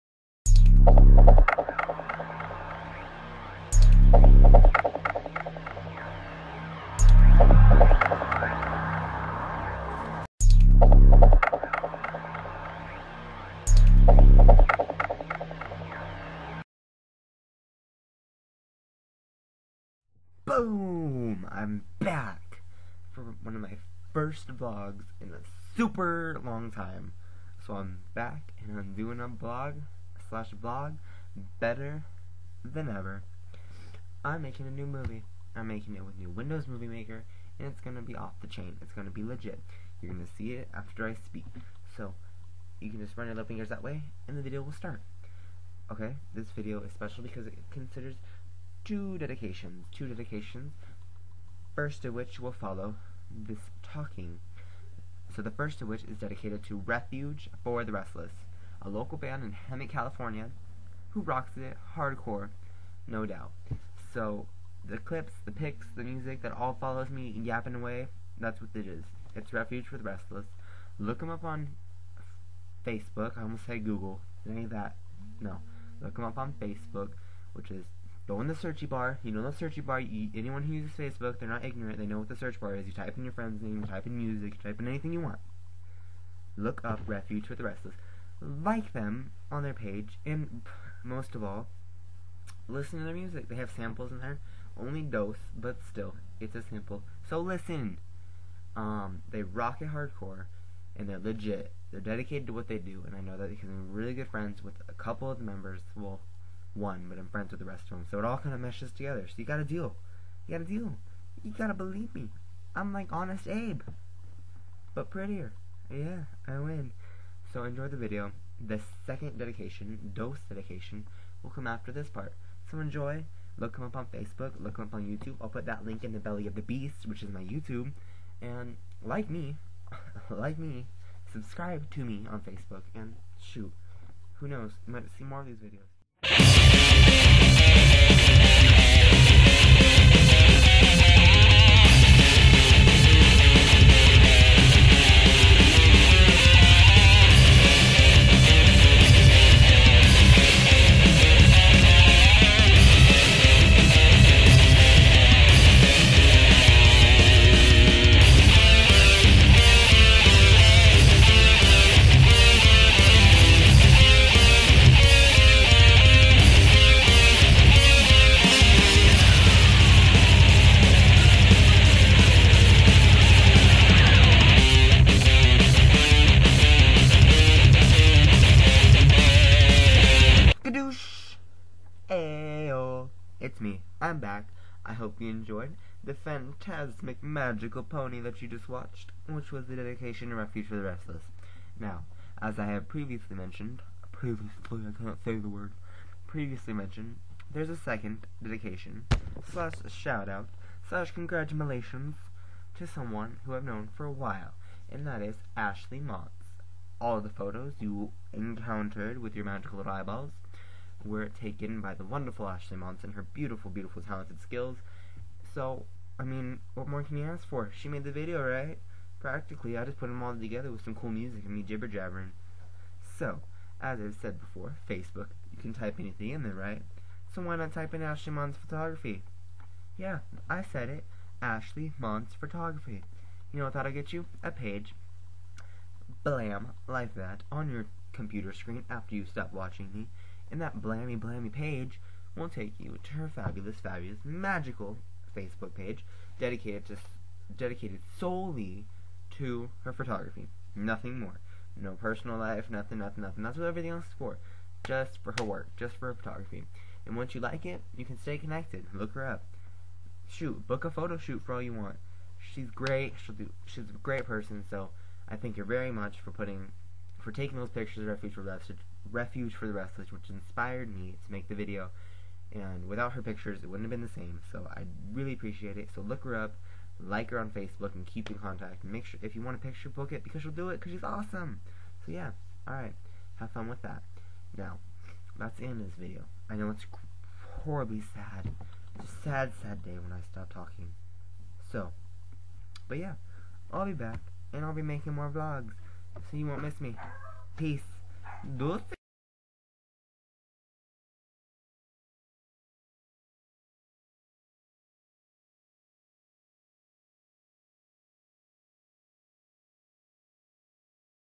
I'm back! Love it or Hate it?
I am back and stronger than ever! I lost my vlog mojo but now nothin can stop meh! This video goes out to Refuge For The Restless and Ashley Montz/ Ashley Montz Photography these our their links!